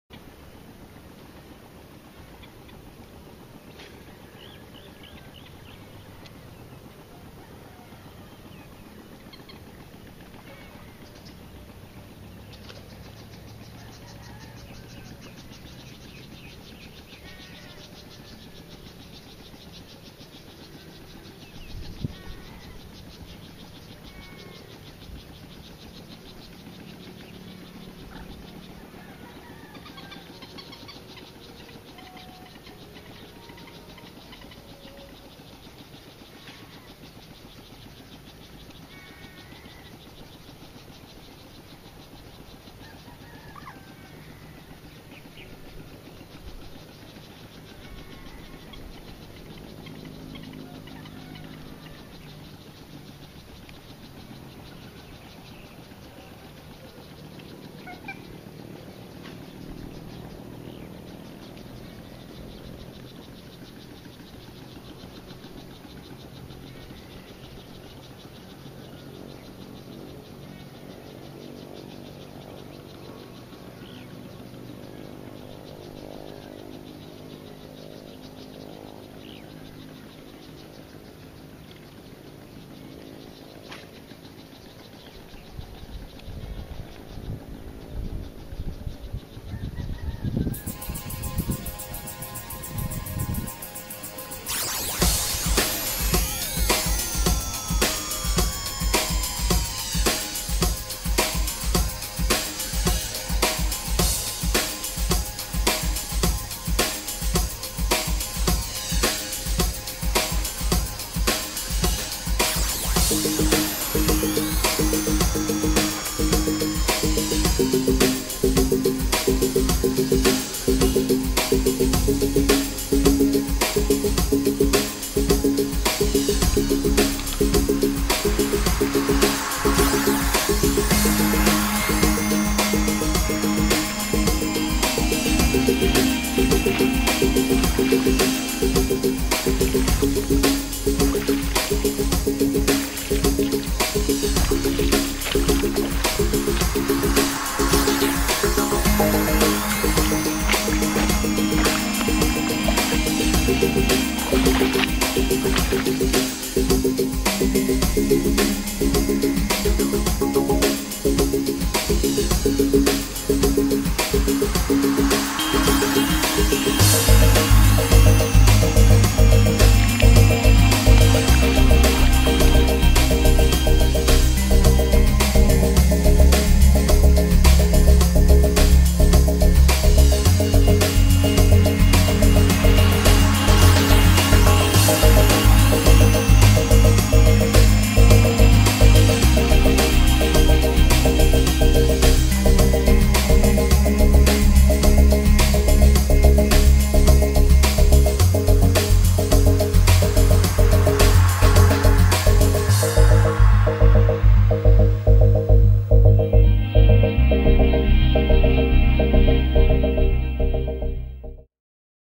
Snakehead Fishing
I went snakehead fishing in my favorite spot. There were a lot of strikes and I managed to land a few. I was using a Kodak Playsport ZX5.